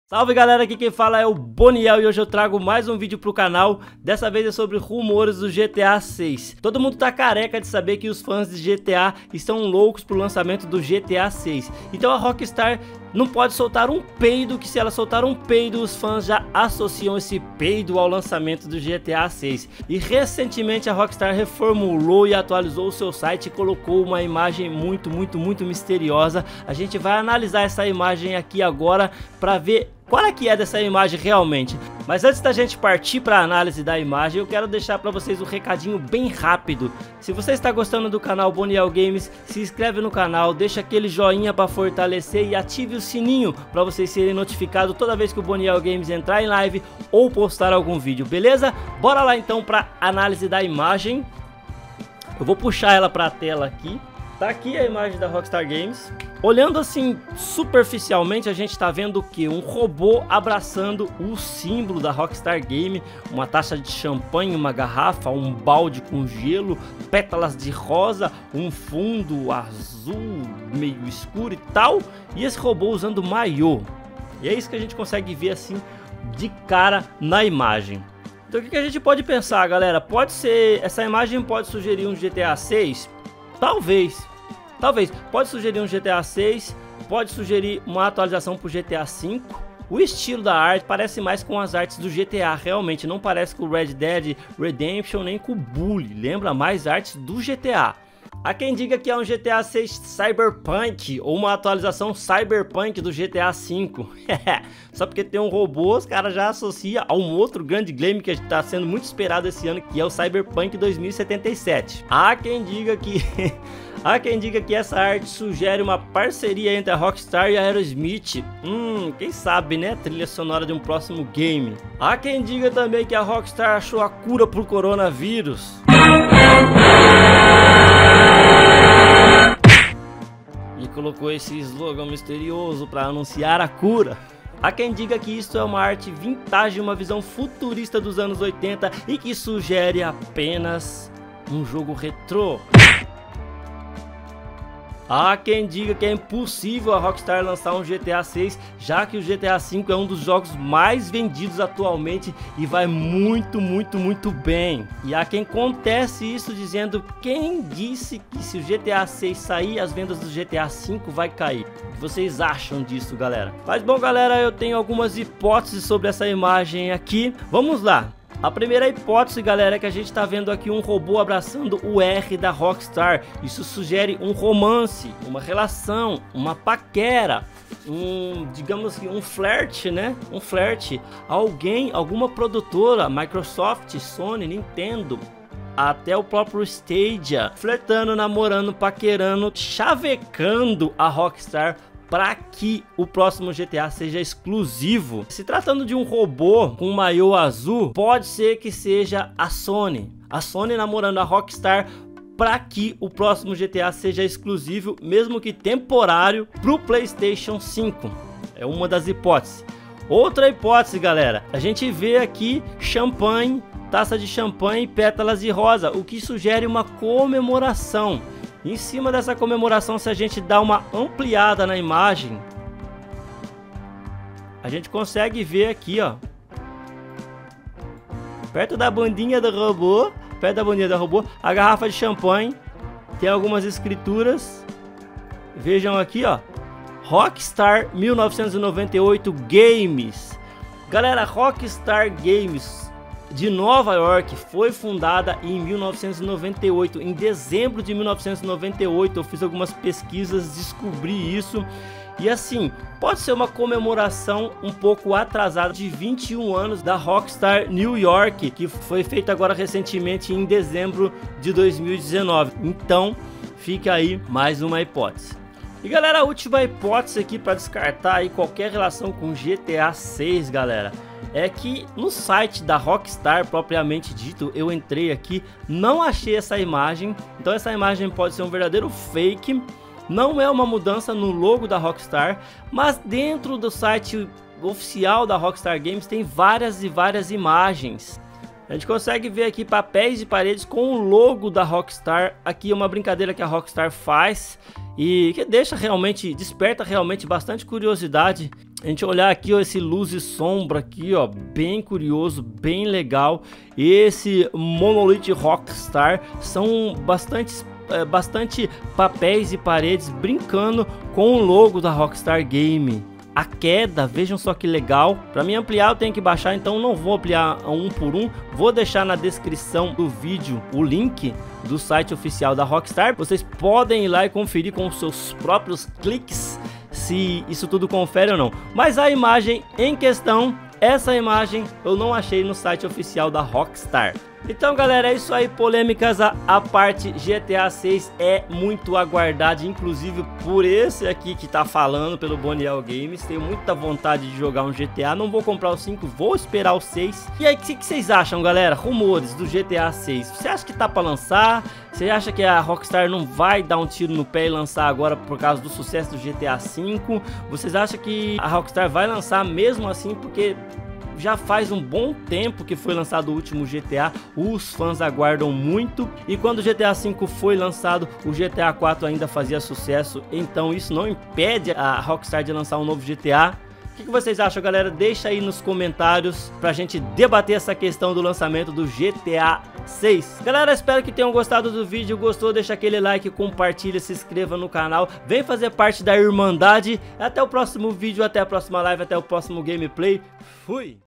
Imagem misteriosa sugere exclusividade de GTA 6 no Play 5.
Imagem misteriosa sugere exclusividade de GTA 6 no Play 5, confira no vídeo tudo que rola por trás dessa imagem misteriosa.

Doe suas moedinhas para ajudar o canal a crescer!

Conheça a loja do nosso parceiro Renato e compre produtos personalizados na R&M:

MUITO OBRIGADO!